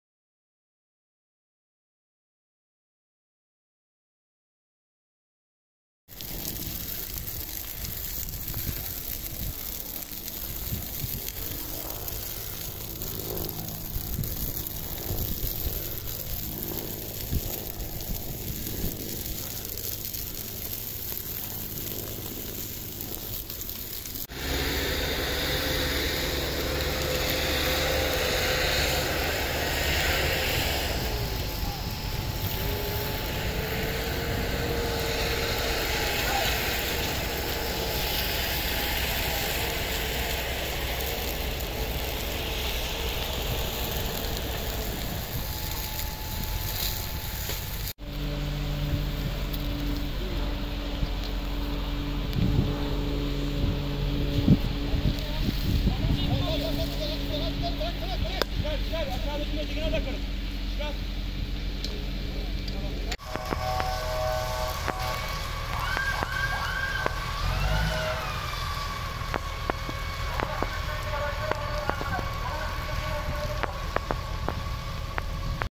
Bayramiç'te Orman Yangını Çıktı
Çanakkale'nin Bayramiç ilçesinde ormanlık alanda çıkan yangın, havadan ve karadan müdahaleyle kontrol altına alınmaya çalışılıyor.İlçeye bağlı Sarıdüz köyündeki ormanlık alanda henüz bilinmeyen nedenle yangın çıktı.